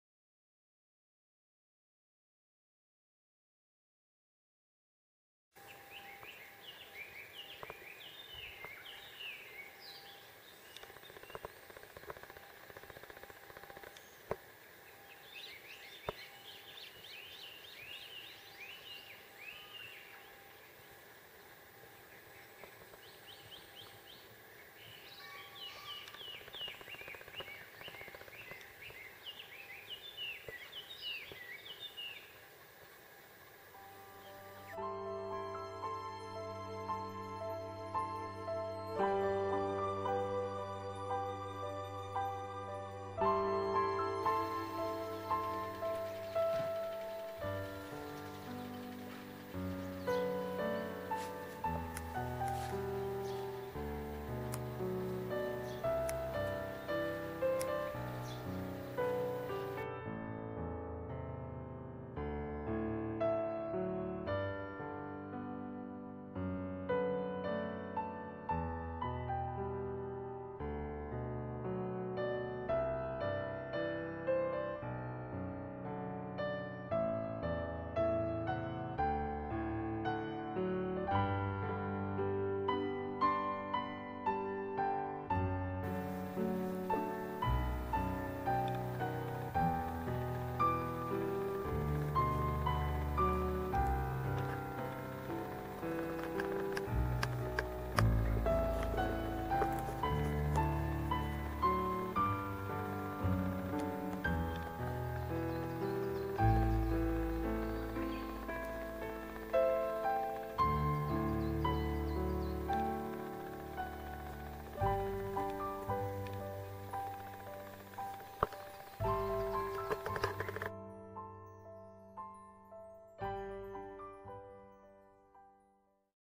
ニリンソウ咲く 霊山
2017年5月5日(金)撮影。福島県伊達市の霊山(りょうぜん)。この時期 霊山寺跡では、ニリンソウの群落を見る事が出来ます。